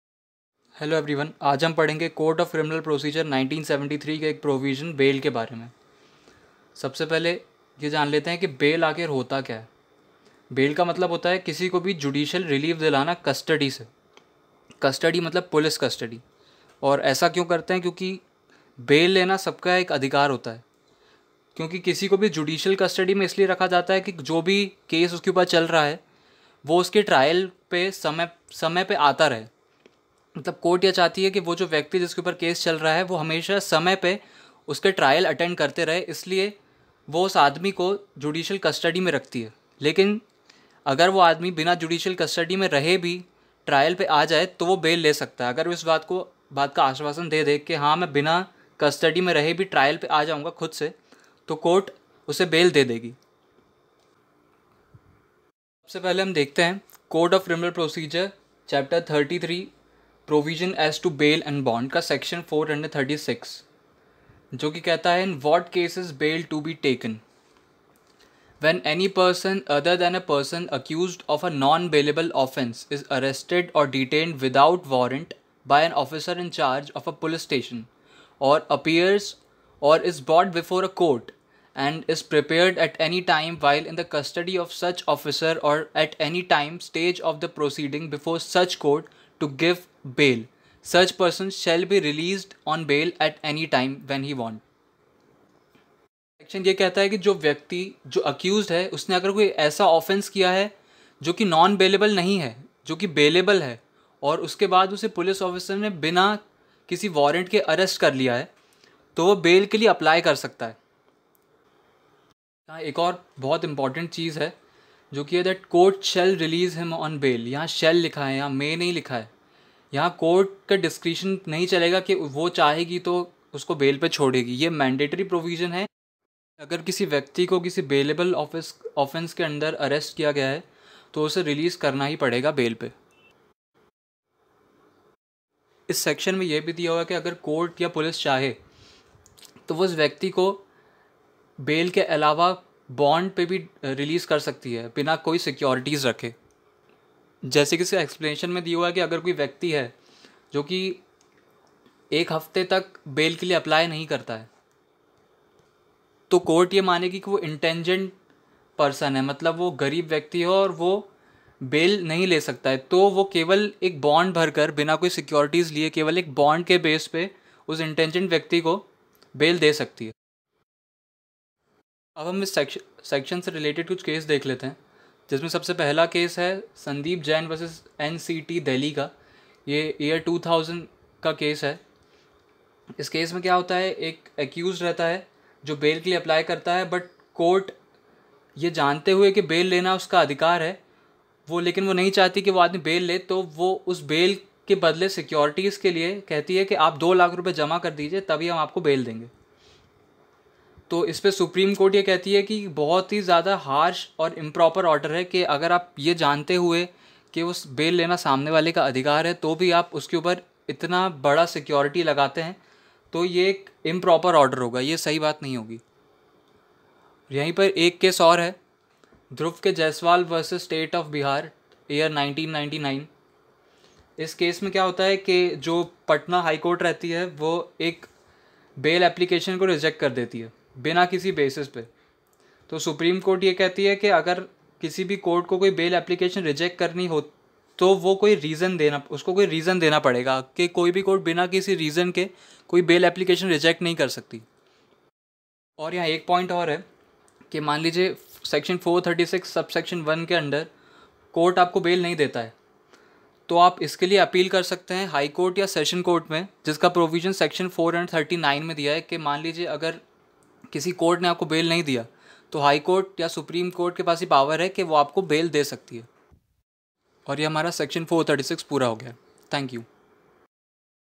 Section 436 CrPC In what cases bail to be taken explained in Hindi and English | how to get bail
Section436CrPC |Section436CrPCIn what cases bail to be taken.